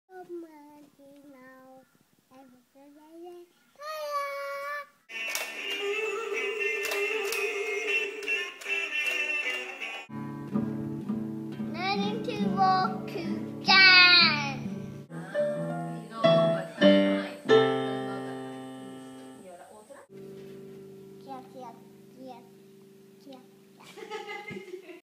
The boy's musical performances (5x5)
This is a 5x5 video (5 shots of 5 seconds each) with some performances by my son.
They are sorted in chronological order, except for the very last shot